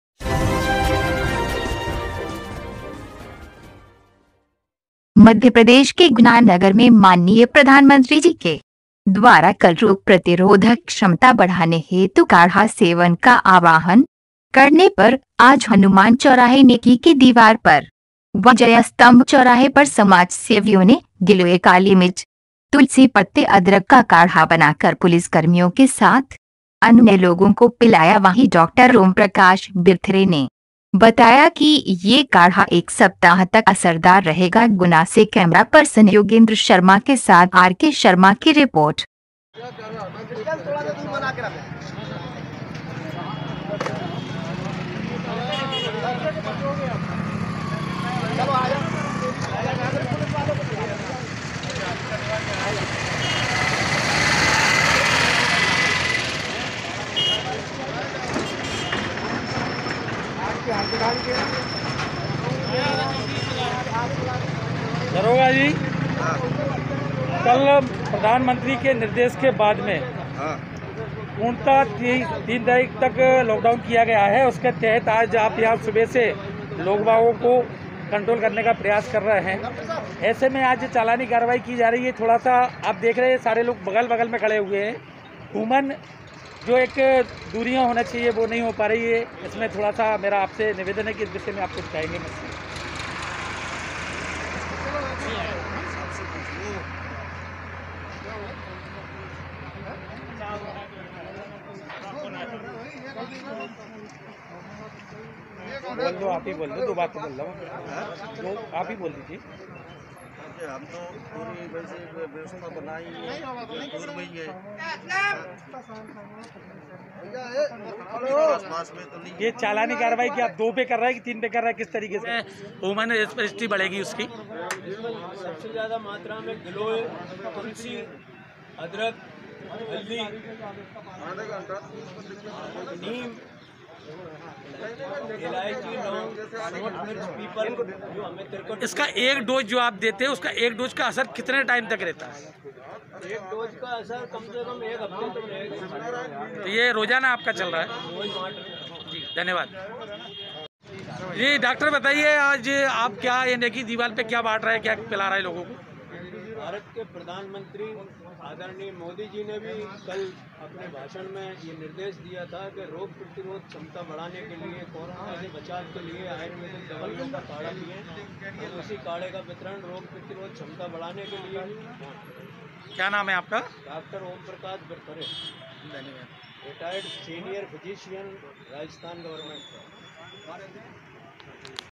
गुना प्रधानमंत्री जी के आवाहन पर रोग प्रतिरोधक काढ़ा पिलाया। MP NEWS CAST
मध्यप्रदेश के गुना नगर में माननीय प्रधानमंत्री जी के द्वारा कल रोग प्रतिरोधक क्षमता  बढ़ाने हेतु काढ़ा सेवन का आवाहन करने पर आज हनुमान चौराहे नेकी की दीवार पर व जयस्तम्भ चौराहे पर समाज सेवियों ने गिलोय कालीमिर्च तुलसी पत्ते अदरक का काढ़ा बना कर पुलिसकर्मियों के साथ अन्य लोगो को पिलाया।वही डॉक्टर ओमप्रकाश बिरथरे ने बताया कि ये काढ़ा एक सप्ताह तक असरदार रहेगा।    *गुना से कैमरा पर्सन योगेंद्र शर्मा के साथ आर के शर्मा की रिपोर्ट*